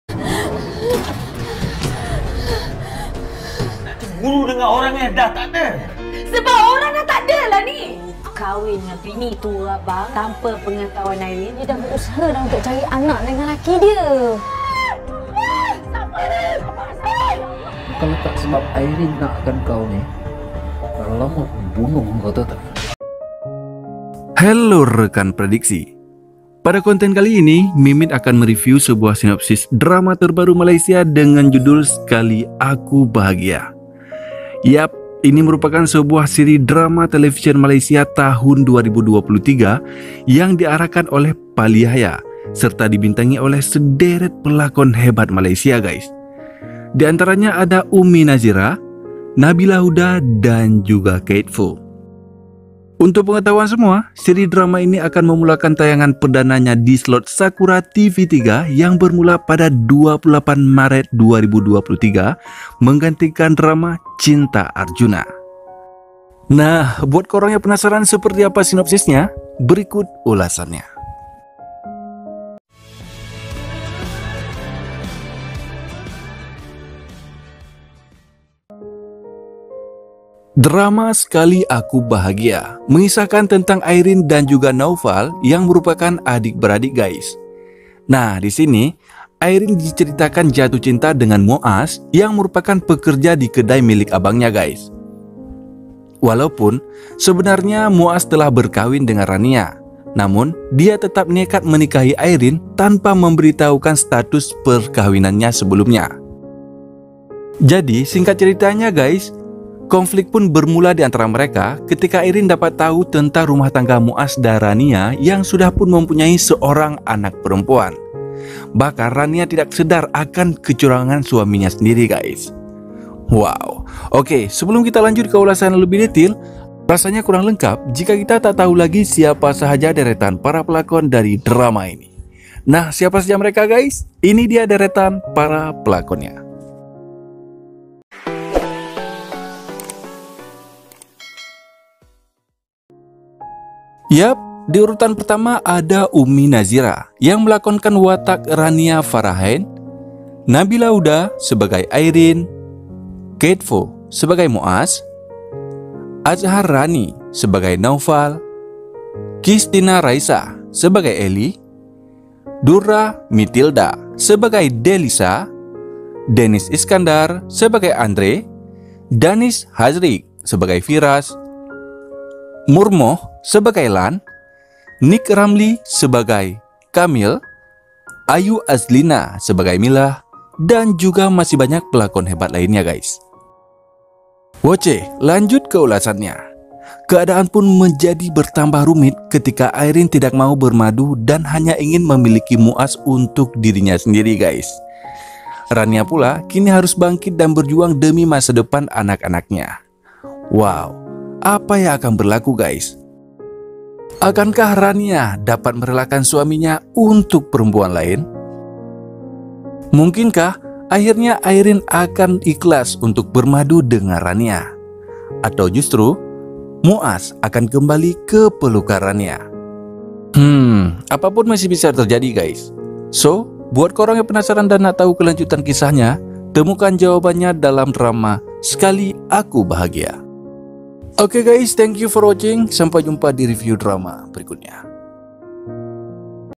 Sinopsis Drama Sekali Aku Bahagia Full Episode
Mengisahkan tentang Aireen yang jatuh cinta kepaa Muaz, seorang lelaki yang bekerja ditempat abangnya yang bernama Naufal. Demi kebahagian adiknya, Naufal merelakan Aireen untuk berkahwin dengan Muaz yang sebernarnya telah menikah dengan Rania dan memiliki seorang anak. Lambat Laun, Aireen dapat tau tentang Rania Isteri pertama Muaz dan membuat rumah tangga mereka mulai goyah. Naufal yang mengetahui adiknya adalah isteri kedua Muaz, dia mengatur rencana untuk menghancurkan Rania yang dianggap peganggu kebahagiaan Aireen. Berikut ulasannya